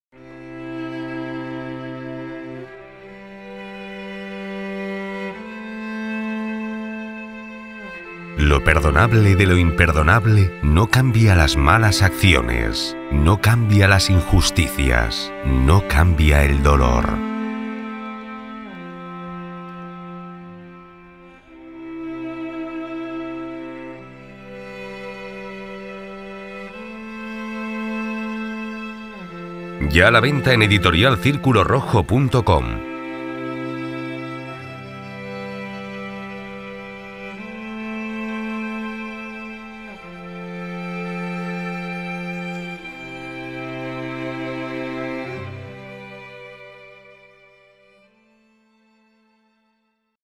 Lo perdonable de lo imperdonable (Booktrailer) - Editorial Círculo Rojo
La inspiración de este libro la dan los hechos acontecidos durante la dictadura militar en Chile, a consecuencia de lo cual, muchas fueron las víctimas, hombres, mujeres, niños, ancianos, sin importar que éstos estuvieran enfermos o las mujeres embarazadas. Familias completas desde el padre, al más menor de la familia, mujeres violadas, hombres torturados hasta la muerte. Muchos de ellos fueron perseguidos injustamente, encarcelados, torturados, desaparecidos y asesinados y todo ello
por el hambre de poder.
Como también las tantas injusticias de la justicia, cometidas con muchas personas inocentes, las cuales son privadas de lo más preciado que tiene todo ser humano: su libertad. Cumpliendo largas condenas, viviendo en forma inhumana en las diferentes
cárceles del mundo, las cuales están sobrecopadas de internos, tanto hombres como mujeres, jóvenes y jovencitas que son débiles, son abusados, convirtiéndolos en mercadería sexual.
Muchos de ellos después de haber pasado por atrocidades de cosas y estar tras las rejas, se comprueba su inocencia, la disculpa es "La justicia también se equivoca".
La justicia jamás se equivoca, los equivocados son, los que hacen cumplir la justicia, En ambas partes el hombre pierde su dignidad humana.